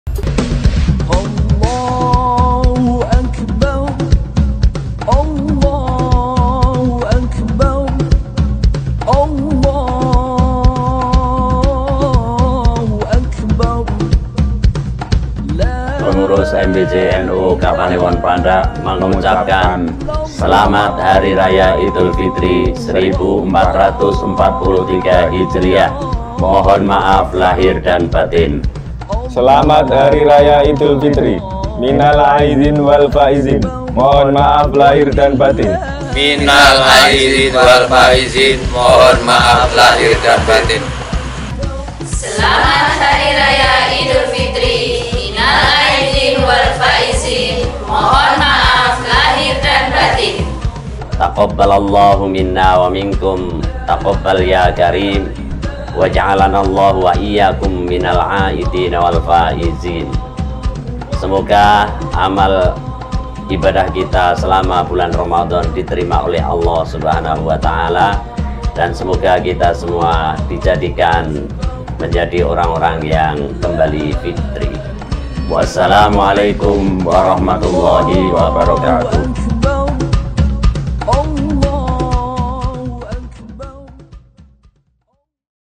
SELAMAT HARI RAYA IDUL FITRI
Ucapan Selamat Idul Fitri oleh Pengurus Majelis Wakil Cabang Nahdlatul Ulama (MWC NU) Kapanewon Pandak, Kabupaten Bantul, Provinsi DIY.

Rais Syuriyah: KH. Ahmad Murod, S.Ag
Ketua Tanfidziyah: Sani Thohari, S.Pd